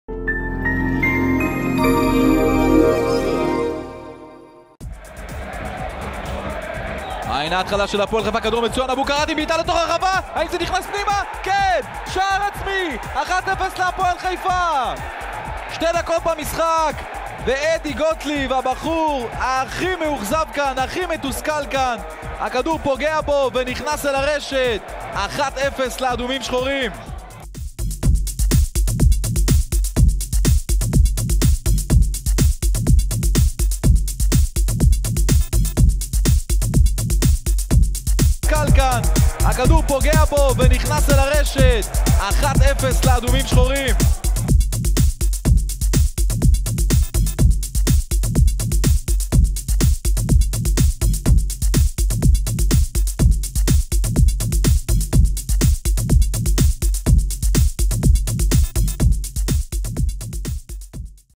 הפועל עכו - הפועל חיפה 1:0, גוטליב בשער עצמי
Hapoel Ako - Hapoel Haifa 0:1, Gotliv with an own goal
ליגת העל, עונת 2012/13, מחזור 24

דקה 2: הפועל חיפה עולה ל-0:1 מהיר! רן אבוקרט קיבל כדור באגף, חדר לרחבה ומסר לרוחב. לרוע מזלה של עכו, הבלם אדי גוטליב שניסה להרחיק, בעט לשער הלא נכון והעלה את האורחת ל-0:1.